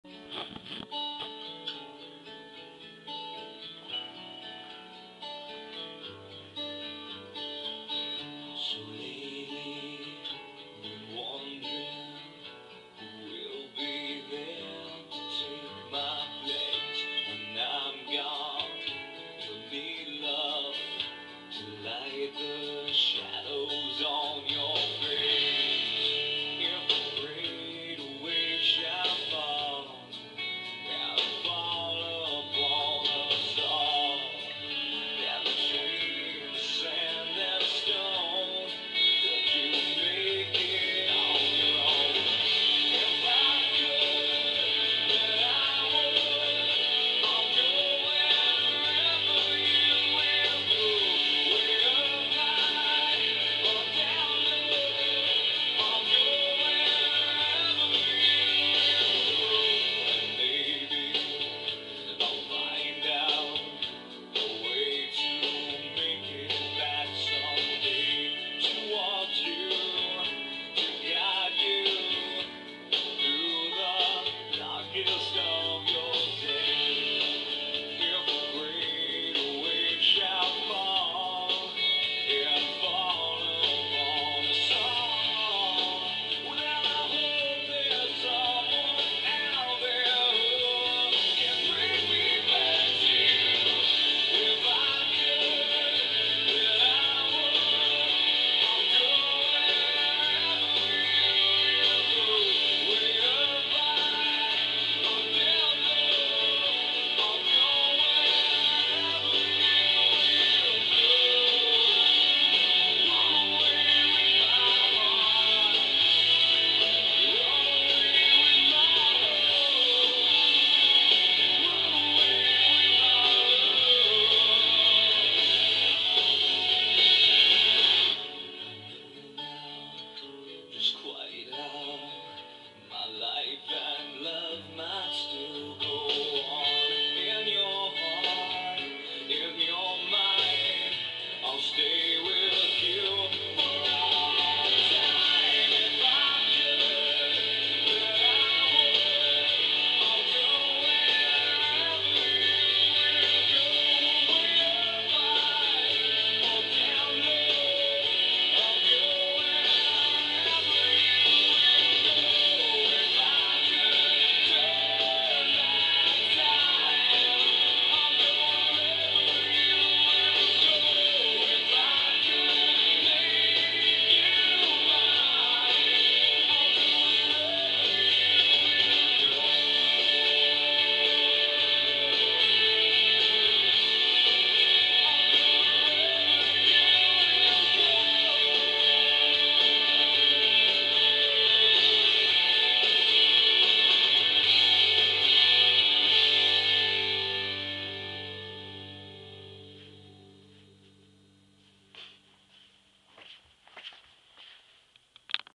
Furry and Pimp Dance
Skrat the Wolf and a Pimp Dancing